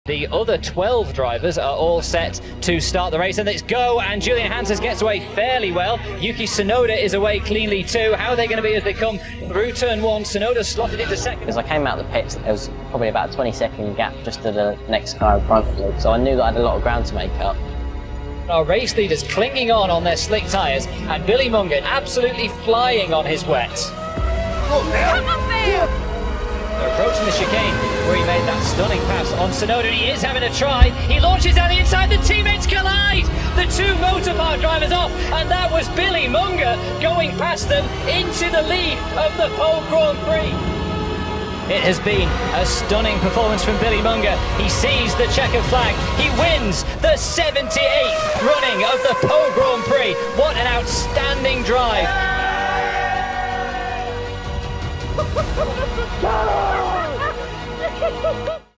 Teen Double Amputee Race Again After Crash | Part 8
The Billy Monger Story | Teen Double Amputee Fights To Race Again

Billy Monger was on the track to becoming a child Formula 4 star. But days before his birthday, Billy suffered a horrendous crash that made headlines around the world and left him in critical condition. Can you imagine your own kid in the seat of a race car going up to 200 MPH? And Should his Dad have stopped him from getting behind the wheel?